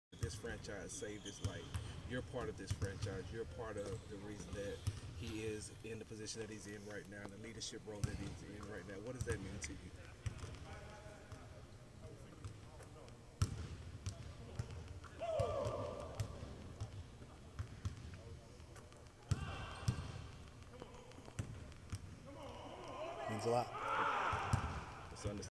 🥲❤️ Stephen Silas gets emotional when he was asked about Kevin Porter Jr.
Houston Rockets head coach Stephen Silas gets emotional when he was asked about Kevin Porter Jr. 🥹

KPJ had said the "organization saved my life, probably."

(via @MarkBermanFox26)